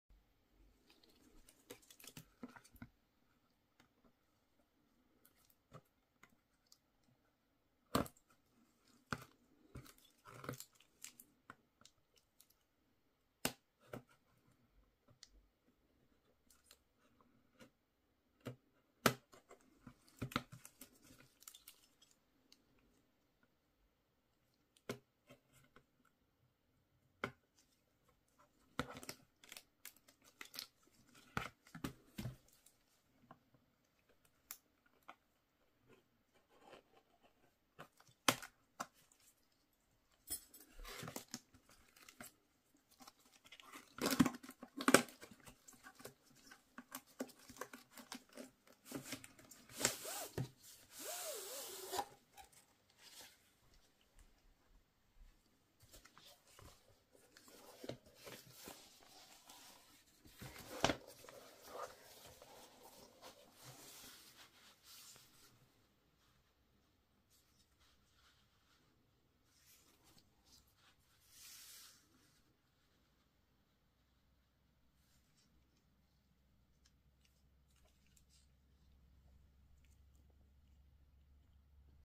Yu-Gi-Oh! [25th Anniversary] - Yugi & Kaiba Quarter Century Game Mat (Unboxing)
Product Information:
Size (24" (W) x 14" (H)) | [61.0 cm (W) x 35.5 cm (H)]
Soft, cushioned rubberized game mat.

Unboxing a Yugi & Kaiba Quarter Century Game Mat. The full video of my opening short but in better quality and without background music. Yu-Gi-Oh! Trading Card Game - Yugi & Kaiba Quarter Century Game Mat [25th Anniversary]. Unboxing like, you ordered something from Wish!

遊戯王カードゲーム

How else can you support me:

You can leave me feedback ...

0:00 Opening
0:48 Game mat